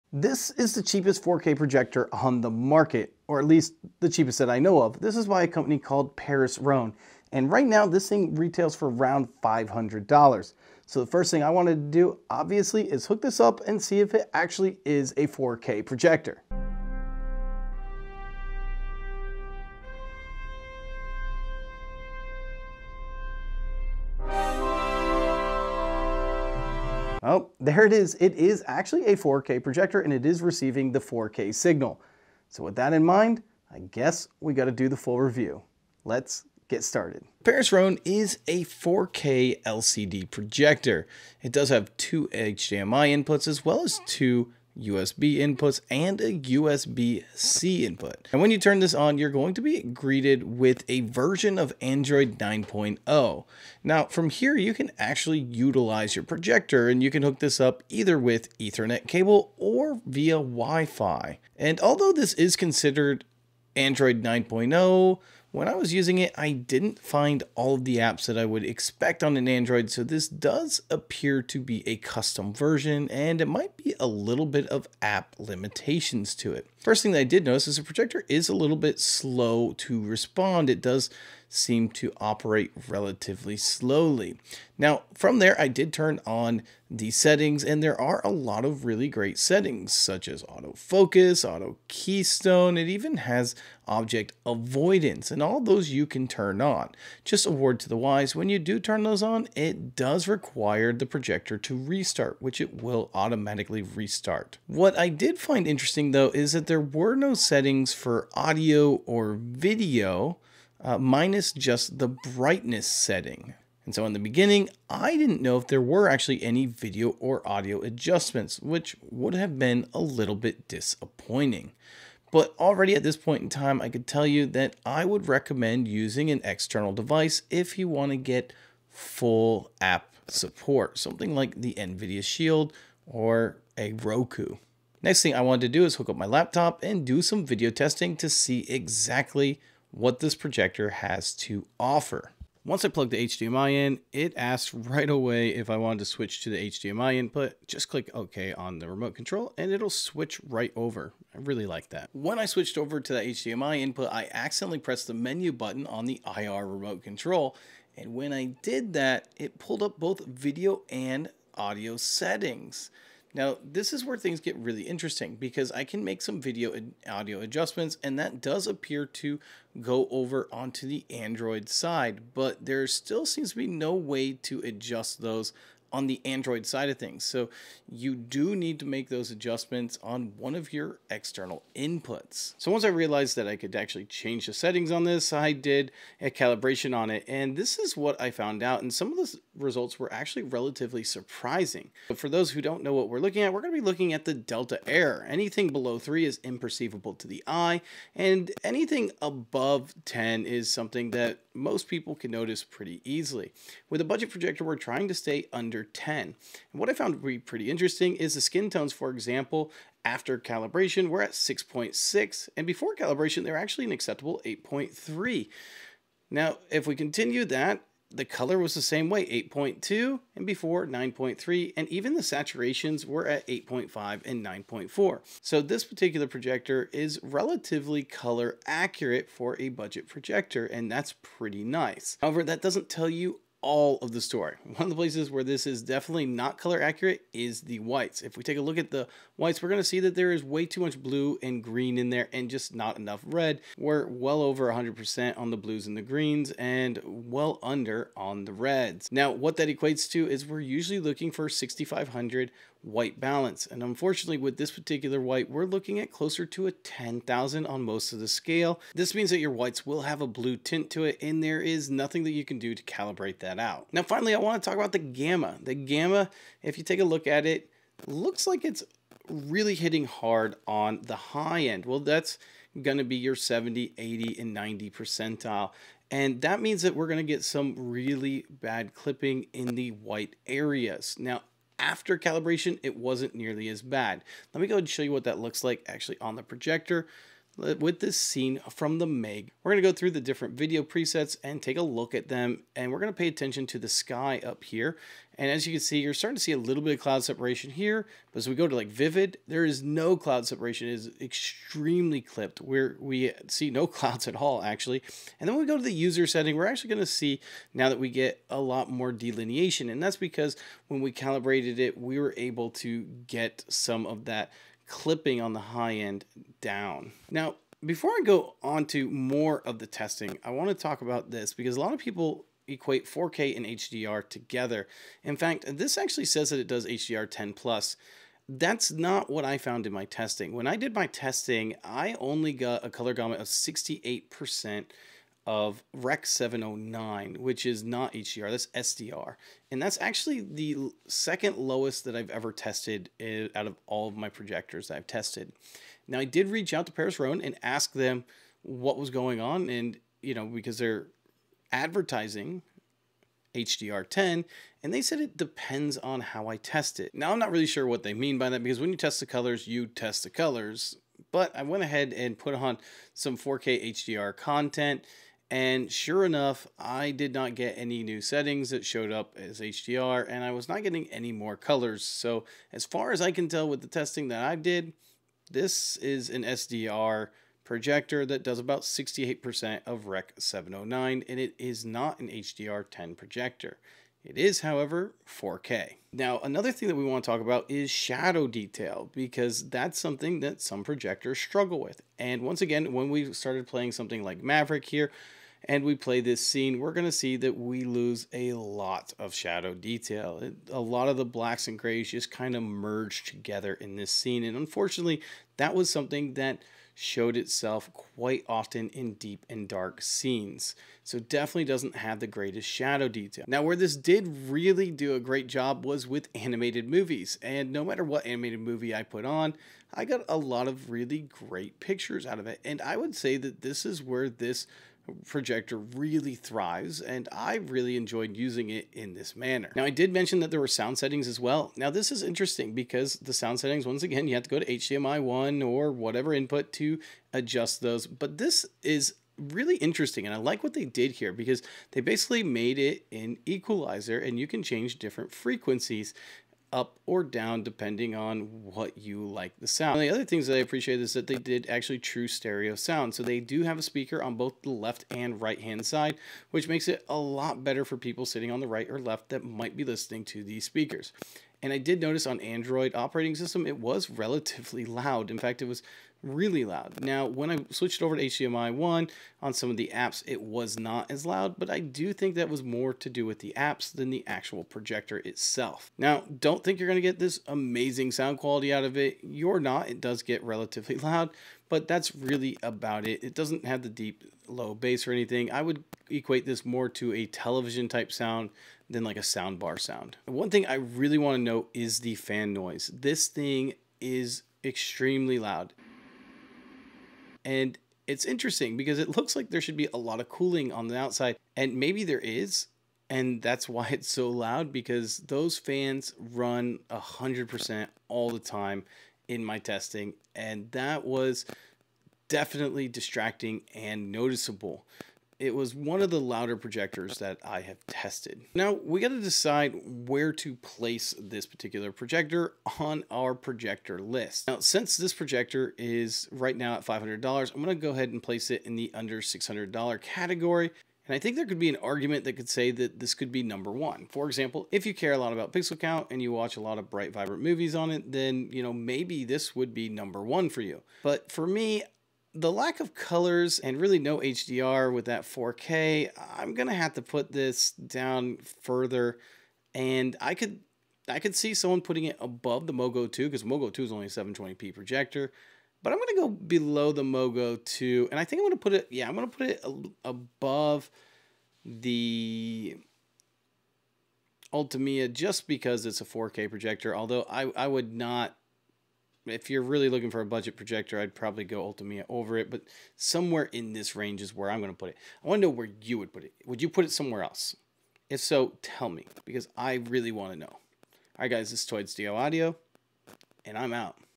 A 4K Projector for $500??? Paris Rhone Sp005 Review
4K projectors are expensive...until now!  The new Paris Rhone SP005 is a 4K projector for only $500!  But will this fill your needs? Take a look as we do in depth testing and review on The cheapest 4K projector on the market - The Paris Rhone SP005.

Worldwide: Amazon

4k? 0:00
Overview 0:37
Color Accuracy 3:02
HDR? 5:17
Shadow Detail 6:27
Animation Domination 6:52
Sound 7:12
Fan Noise 8:12
Competition? 8:42